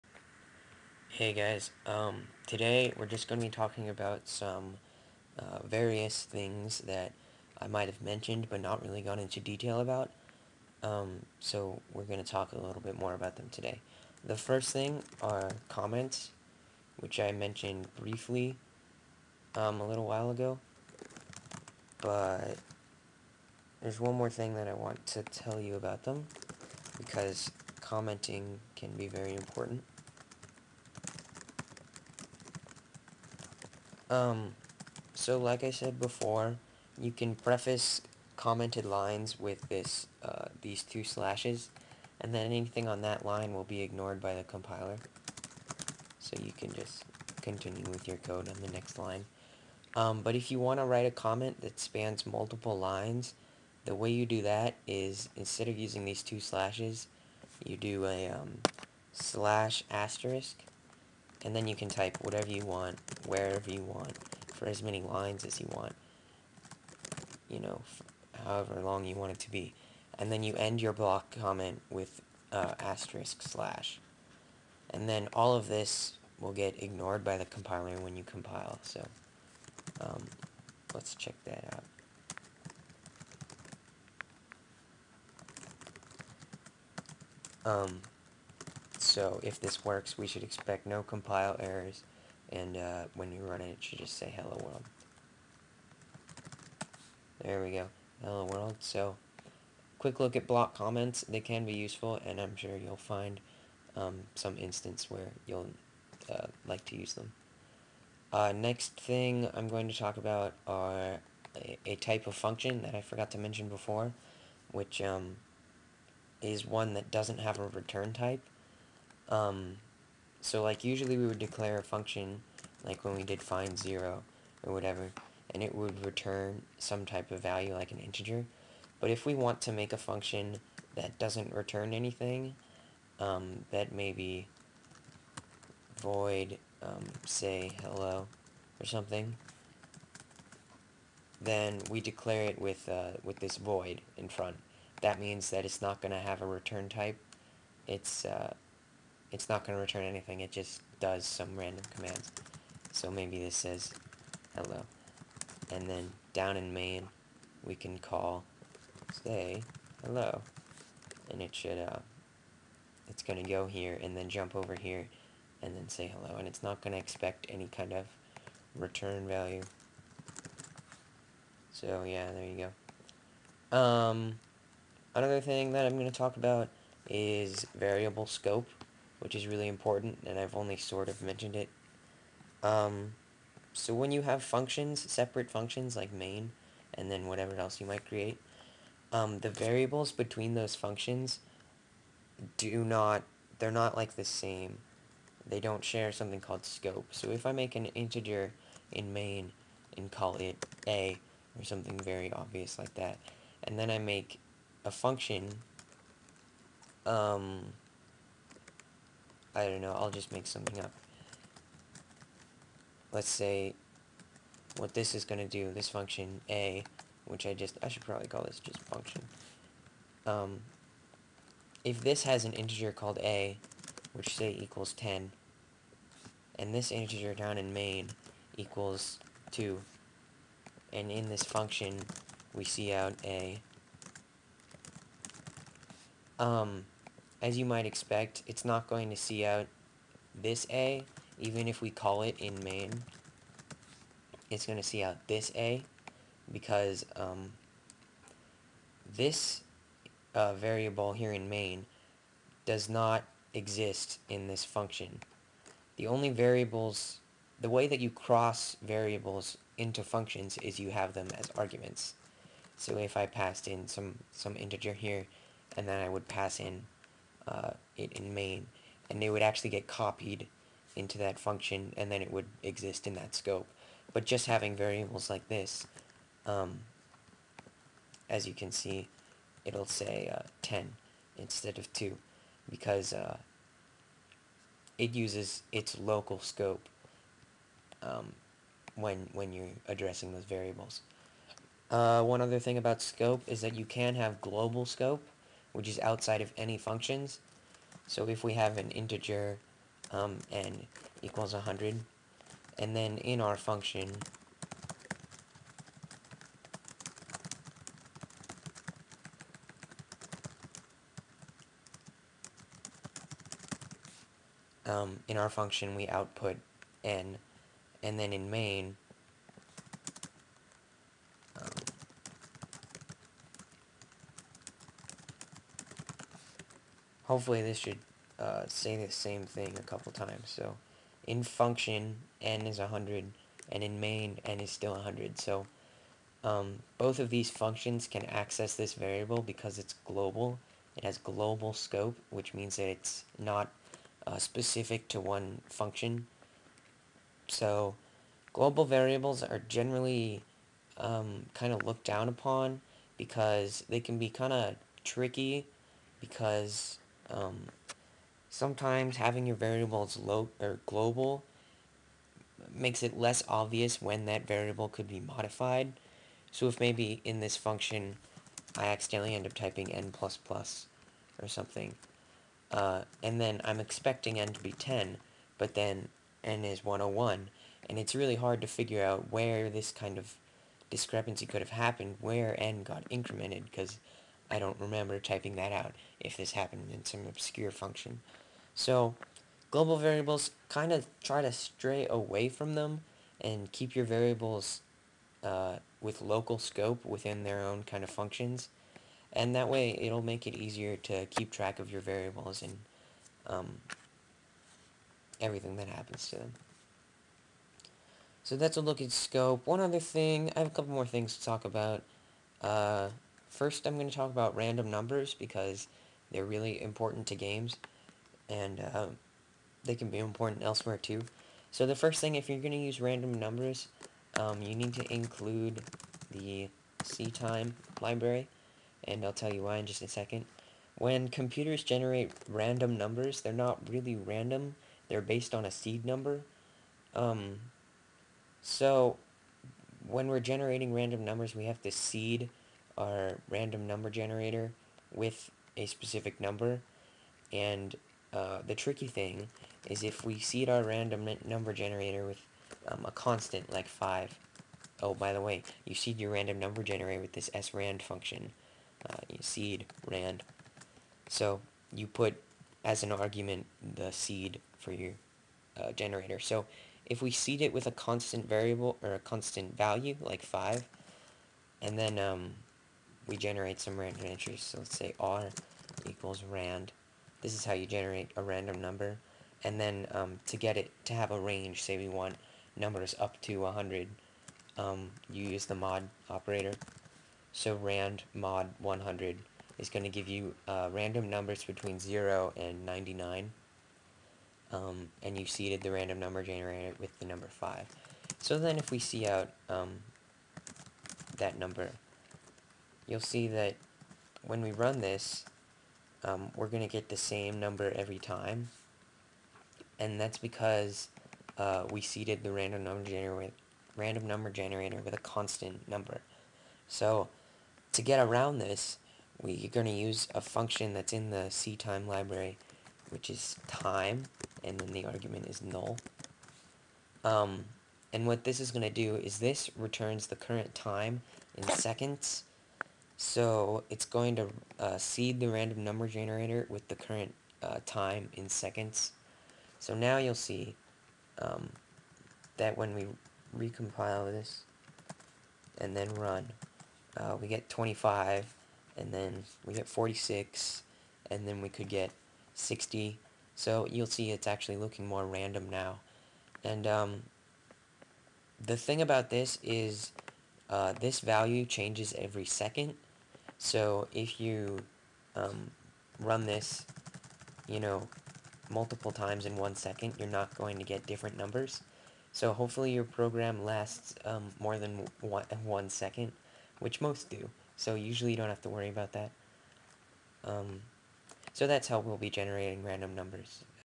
C++ Basic Skills: Lesson 06 "Loose Ends" (part 1 of 2)
A look at some features of C++ I've mentioned before but not discussed much.